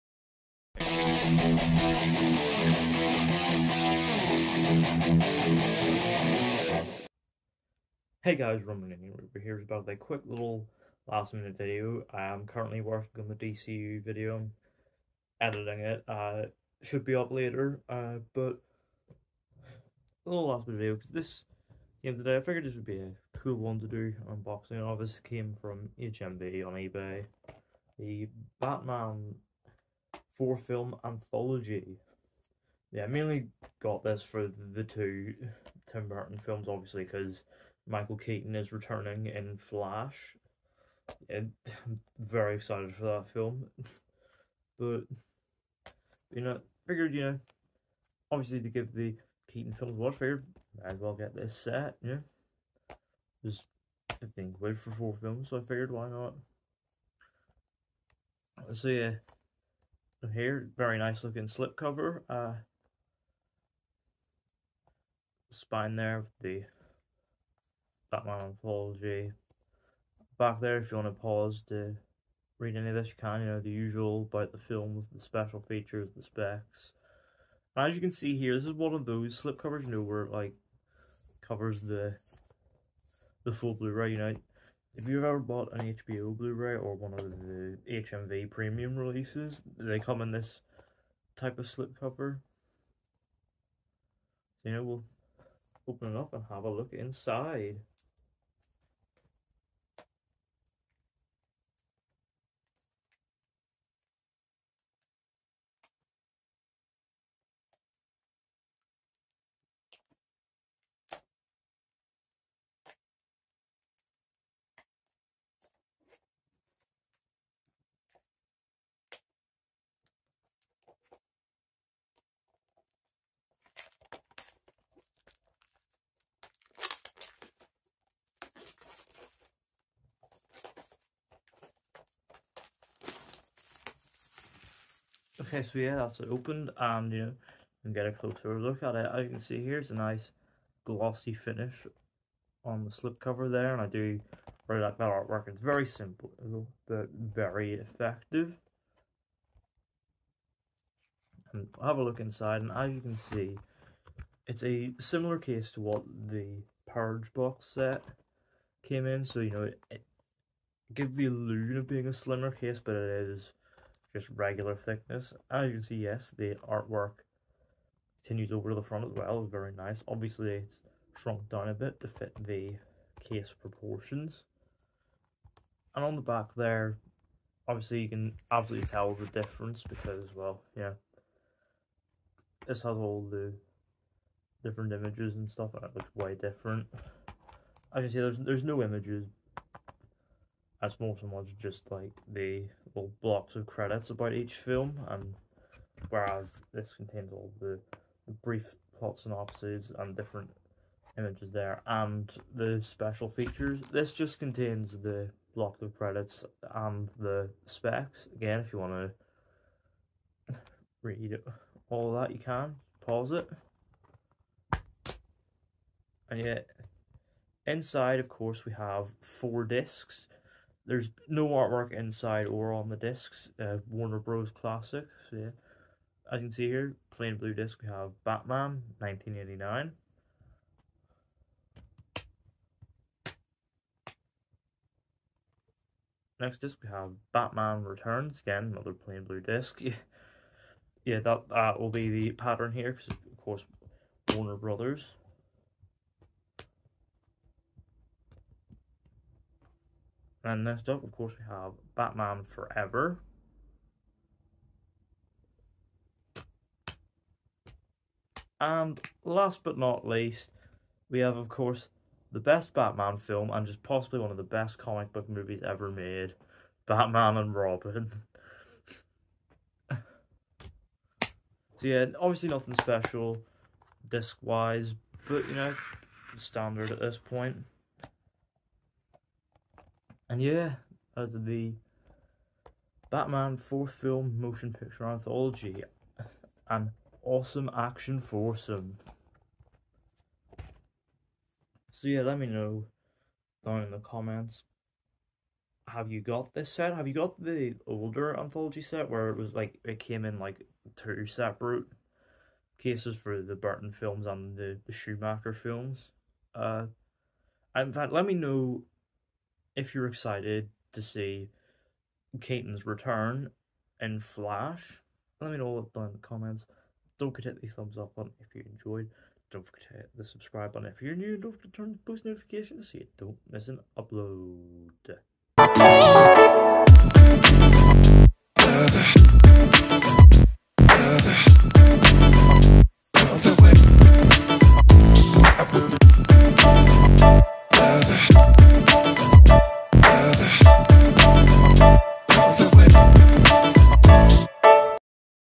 Batman: The Motion Picture Anthology - Blu-ray unboxing
I unbox the Batman Motion Picture Anthology box set on Blu-ray, which includes both the Tim Burton films and both the Joel Schumacher films, to show you what you get for your money.